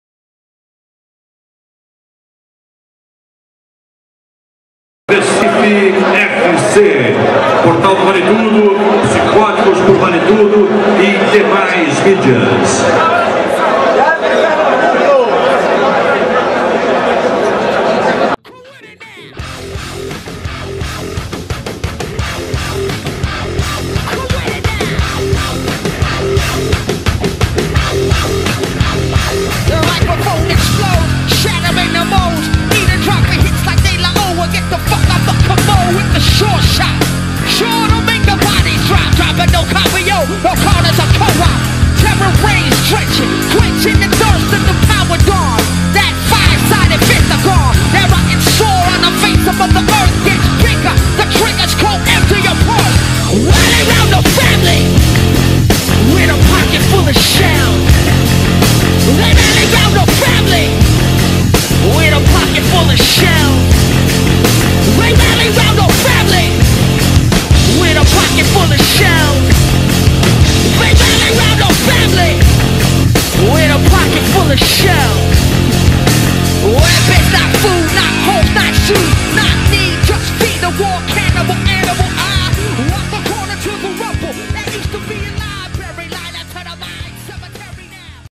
II RFC3 II Melhores momentos
Realizado no Chevroltet Hall - RECIFE - PE, 2 de dezembro 2010

Fotografia: Rafael Maia
Imagens e Edição: Ernesto jr.